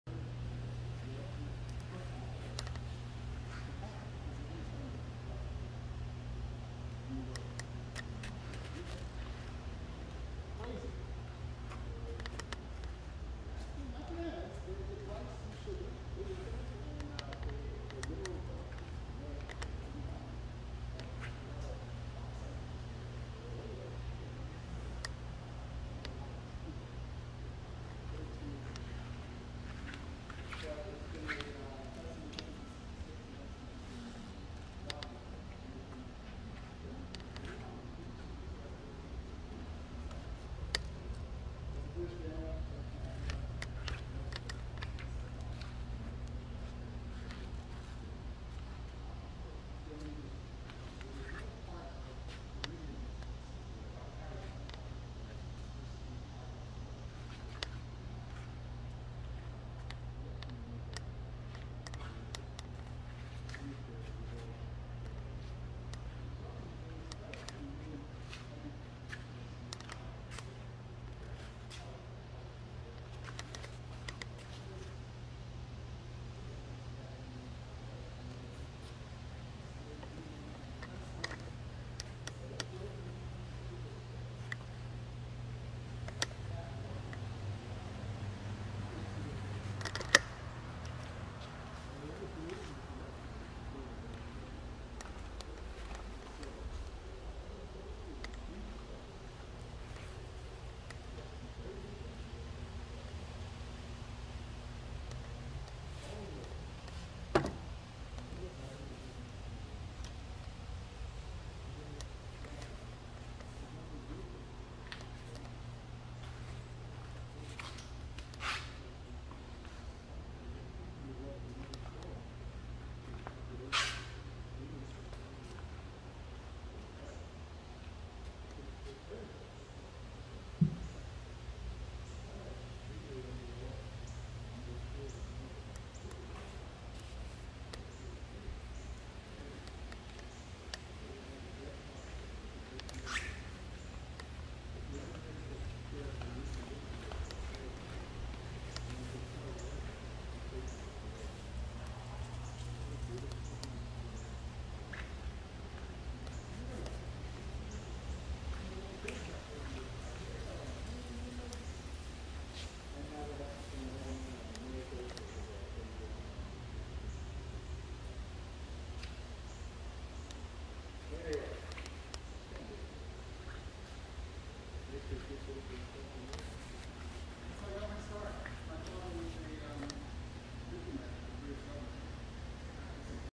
1967 Alfa Romeo Duetto - Walk around views of the exterior and interior and of removed parts
This car will be auctioned on Bring a Trailer.  It was completely disassembled and placed in storage for more than 10 years.